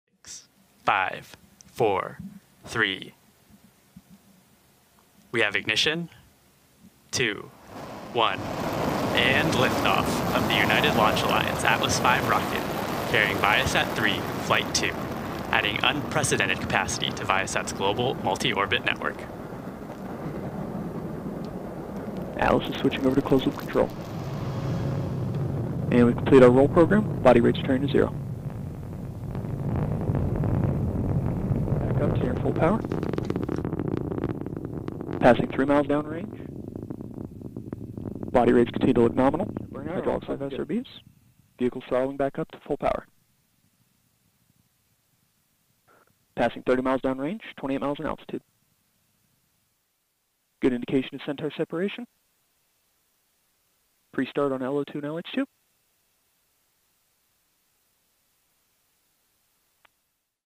Watch ULA's Atlas V 551 Launching ViaSat-3 F2 to Space!
ULA launched Atlas V 551 from SLC-41, Cape Canaveral SFS, Florida, USA on Fri Nov 14, 2025 at 15:04 UTC.

Atlas V 551 configuration launchedViaSat-3 F2, part of a constellation comprised of three Ka-band satellites, each designed to be capable of rapidly shifting capacity throughout its coverage area to deliver bandwidth where and when it’s needed most.

All the rights from this video belong to ULA. This video has been made with informative & journalistic purposes.